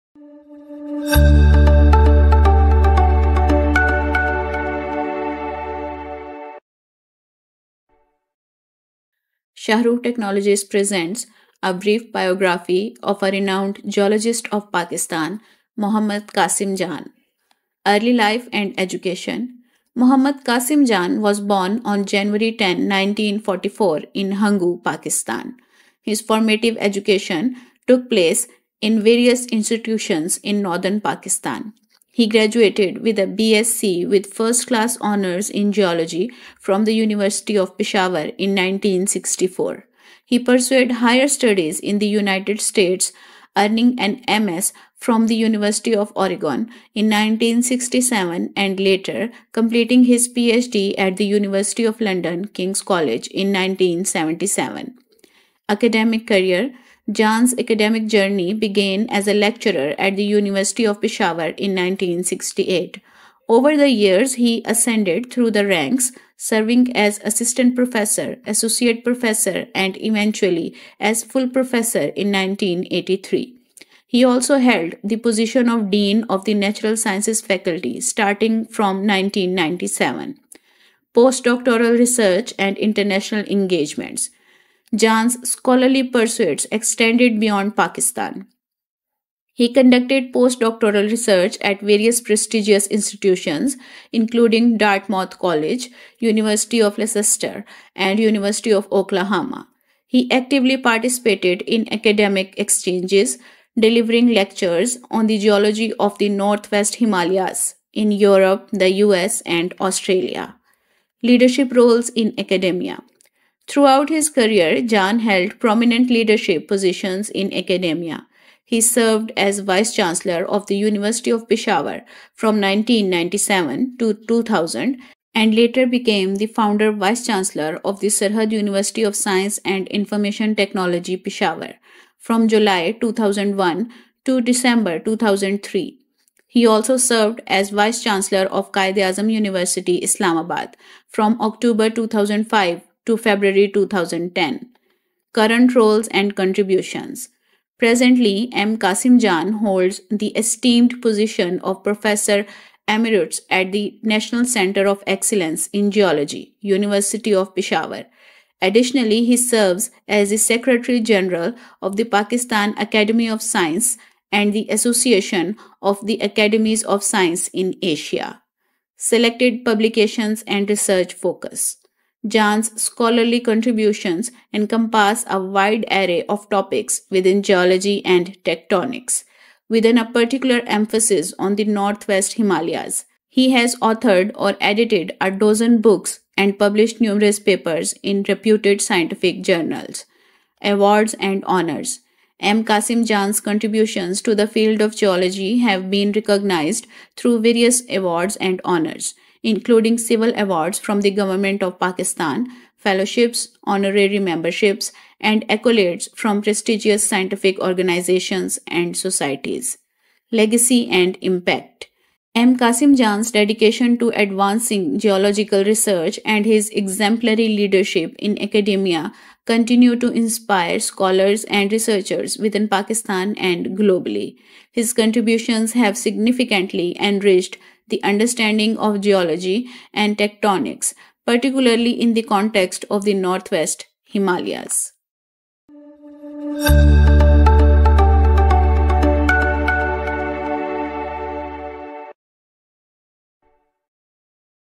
Exploring the Legacy: Biography of Dr. M. Qasim Jan | Shahruh Technologies
Uncover the extraordinary life of Dr. M.Qasim Jan, a distinguished research scientist hailing from Pakistan. 🇵🇰 Join us as we explore his remarkable contributions to the field of science and innovation.